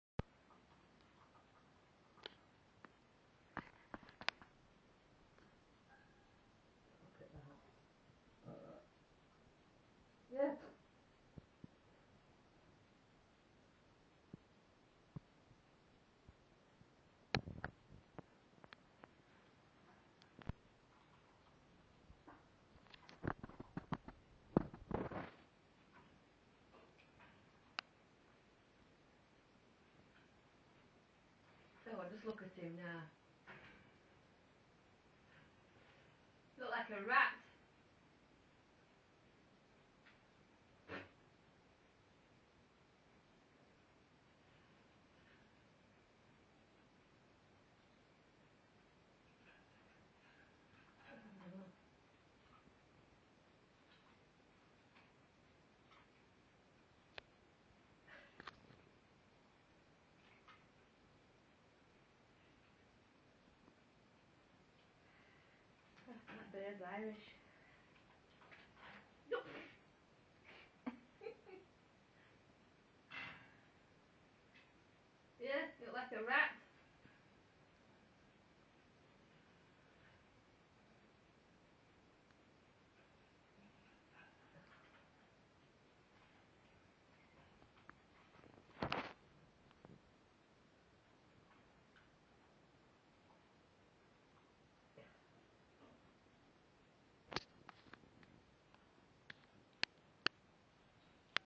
stupid peanut brain budgie
"Joey" our budgie is attempting to take a bath out of his water tub instead of his bath